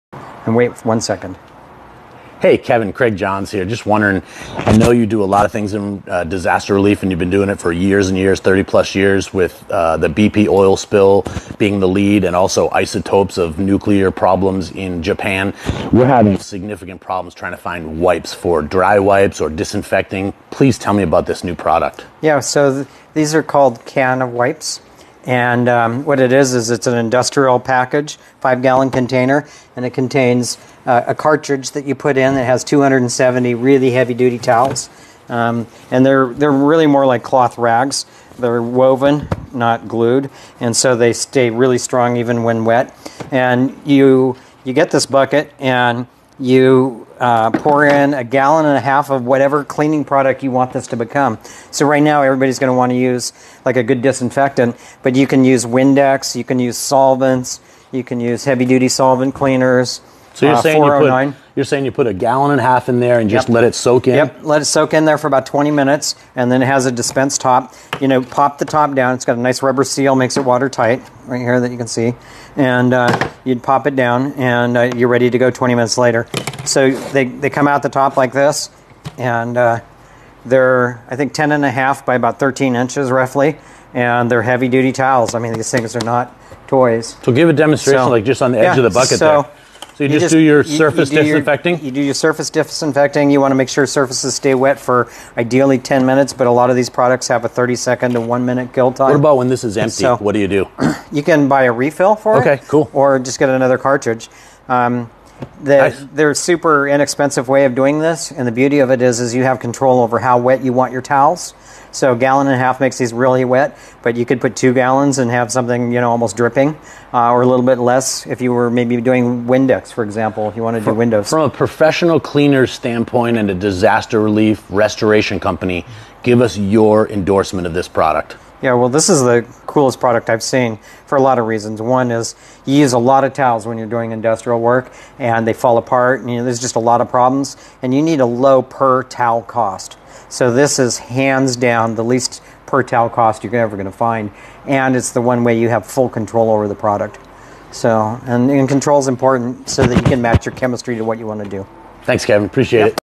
canofwipes.com dry wipe Covid19 disinfecting Tech review by KW (Kevin Wang DCU Disaster Clean Up)
Kevin Wang reviews canofwipes.com industrial cleaning wipes.  Dry wipes system for disinfecting surfaces - cavicide pdi sani wipes clorox lysol Covid